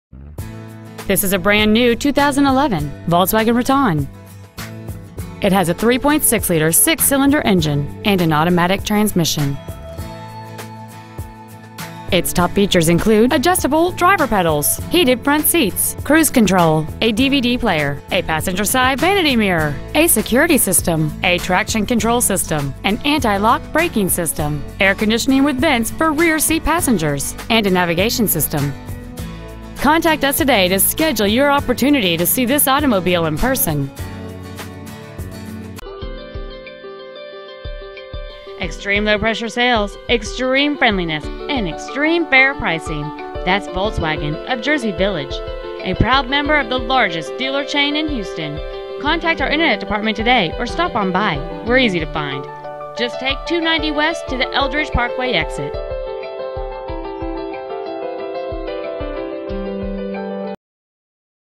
2011 Volkswagen Routan Houston TX 77065
Year : 2011

 Make : Volkswagen

 Model : Routan

 Engine : 3.6L V-6 cyl

 Trans . : Automatic

 Exterior : Twilight Gray Metallic

 Miles : 3

 Interior : Aero Gray

 19550 Northwest Frwy

 2011 Volkswagen Routan Houston TX